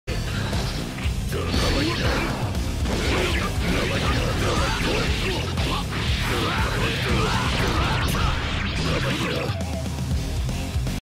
Side switch throw RRC combo
Mostly just because.

Throw RRC IAD delayed j.D 6[H] bt.S 214S 5P 5K cl.S (jc) j.K j.H j.236H